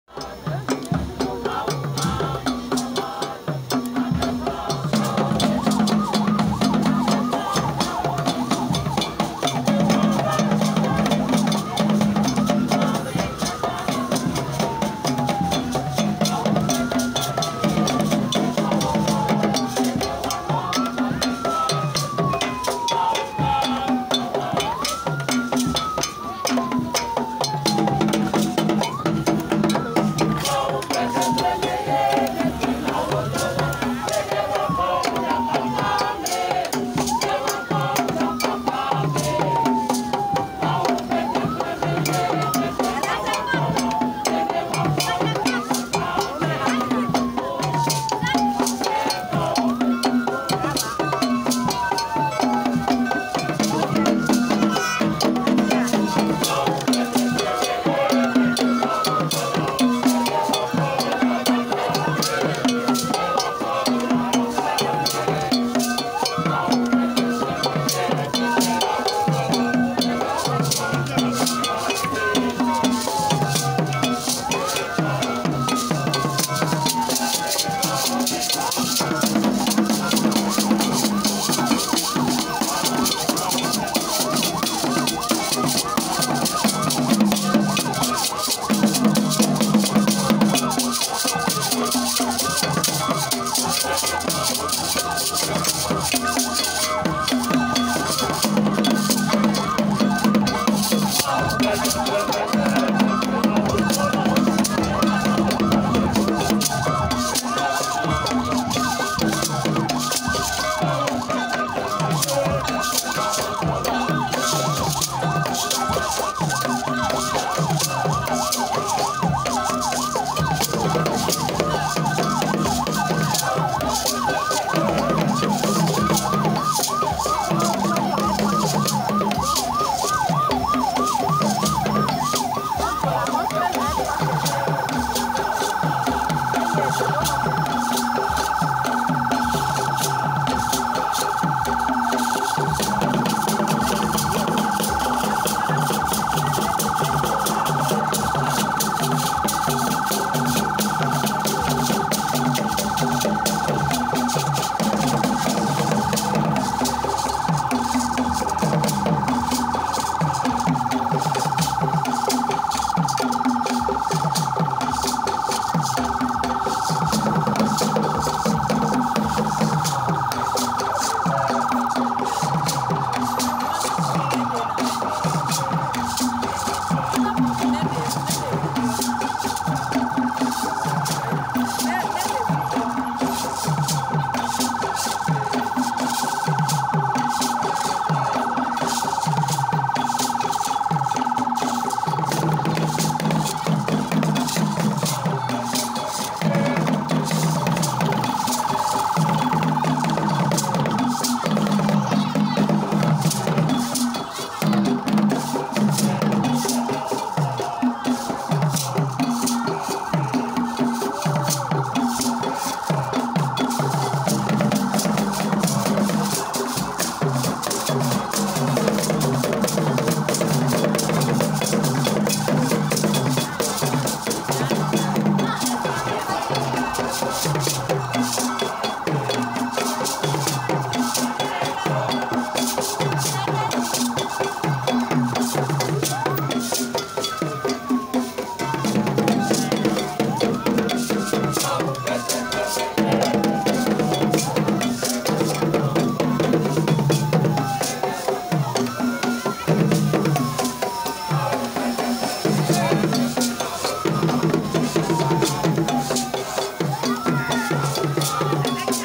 Procession by Dumedefor Choir
At the pre burial service of Rt Rev Dr S. S Agidi